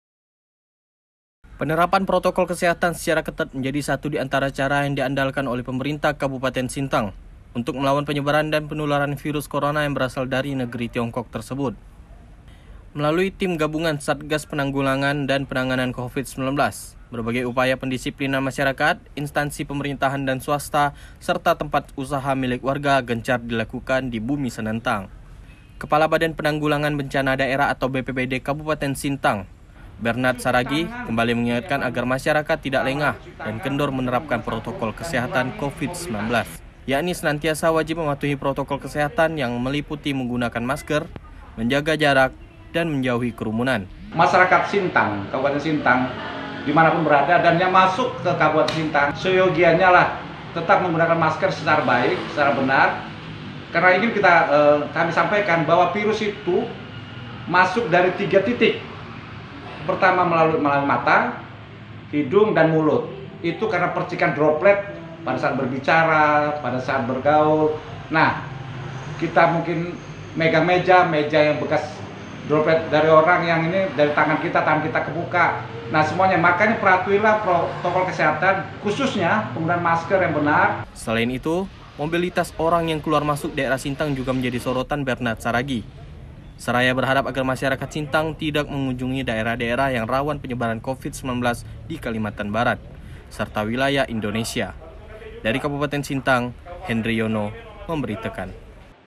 KEPALA BPBD INGATKAN WARGA SIAGA COVID DI BUMI SENENTANG
Belum berakhirnya wabah Covid-19 di Bumi Senentang menuntut masyarakat Kabupaten Sintang harus tetap bersikap waspada. Terutama terhadap penyebaran dan penularan virus corona yang berasal dari Negeri Tirai Bambu tersebut, dengan disiplin menerapkan protokol kesehatan secara ketat